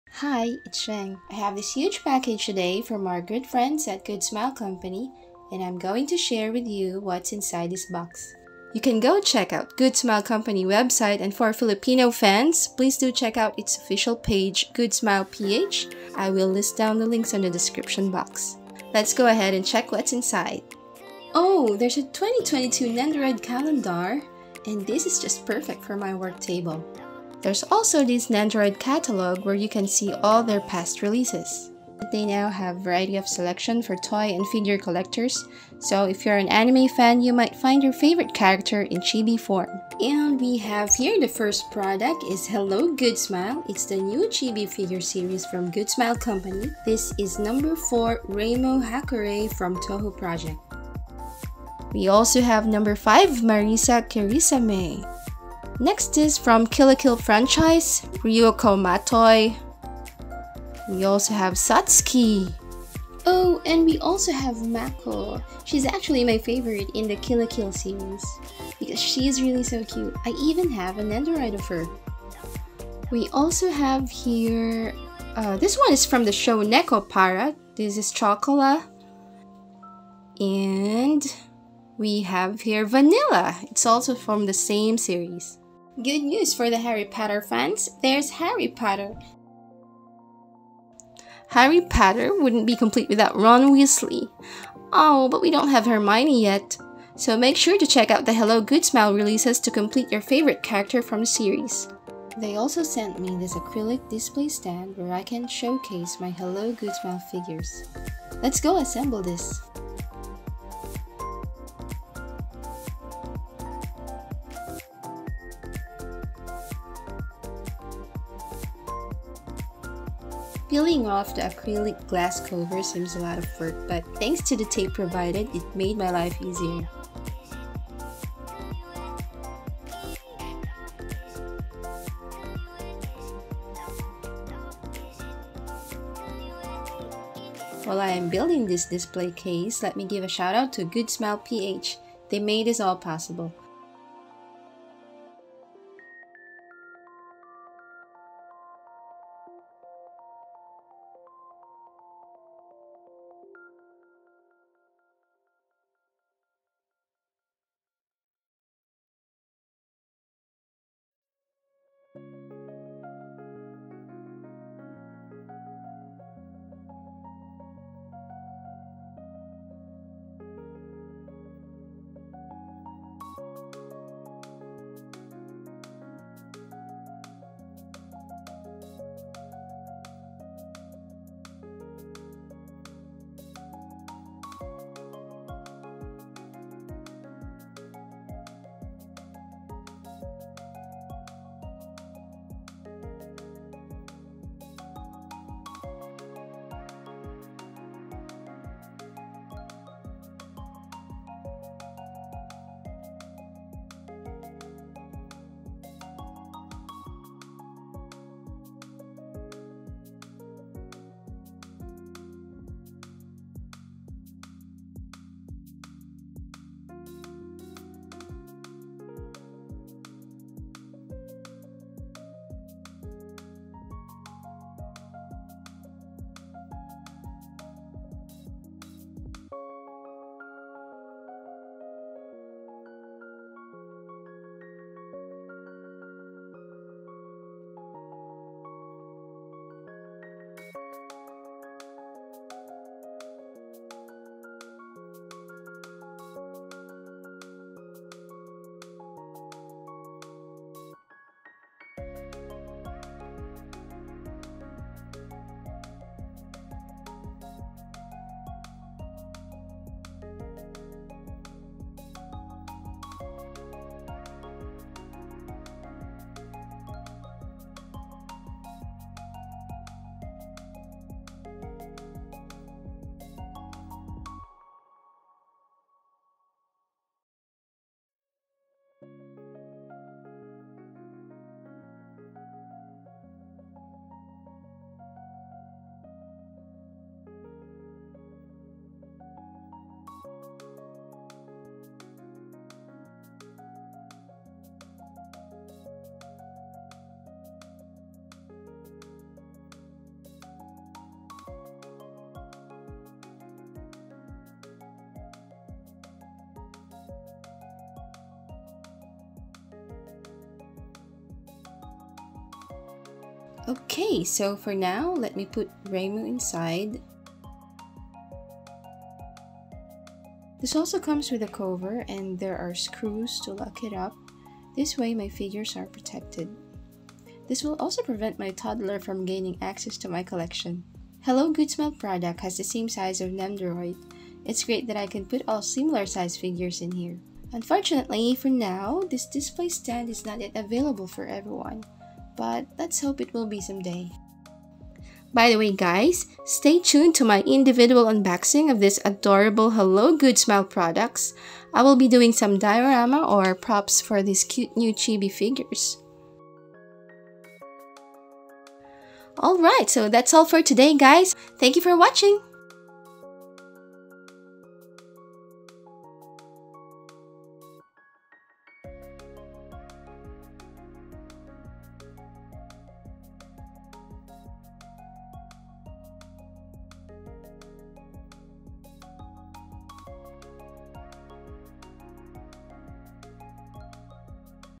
I received a PACKAGE from Good Smile Company! UNBOXING & HELLO! GOOD SMILE Display Stand
Hi guys! I am sorry I have to re-upload this.
I received a special package from GOOD SMILE COMPANY in Japan and I am sharing with you all what's INSIDE the huge package. Big thanks to our good friends in GSC and GoodSmile PH! And YES, It's OFFICIAL I will be working with them on some projects too!

Let's hope that the display case will be available for purchase soon! :)

They have SUPER CUTE hobby products, make sure to check them out on the links:

I also review dollhouses, miniatures, dolls, toys, and anime figures.